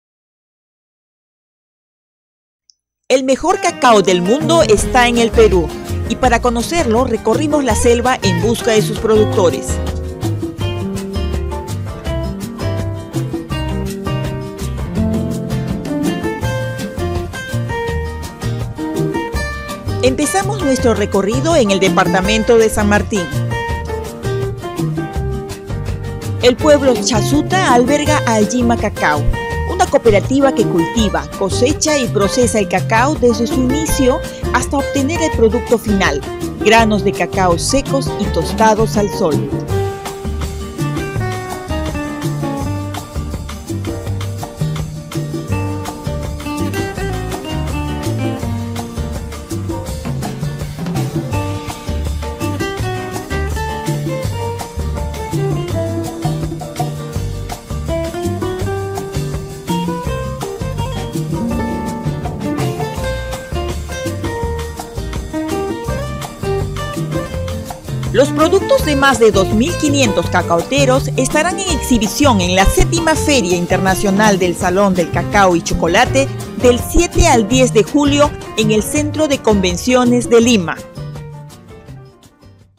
Cooperativa Allima Cacao
¡El mejor cacao del mundo esta en el Perú!

Cooperativa Allima Cacao cultiva, cosecha y procesa el cacao hasta obtener el producto final.